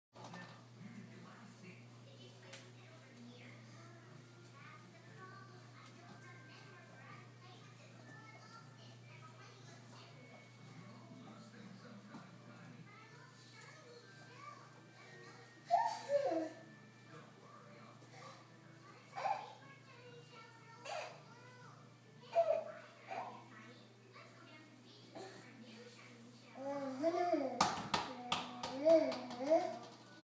1 Year Old Kicking Soccer Ball for 1st Time
1 Year Old Tristan Cameron kicking soccer ball for the 1st time.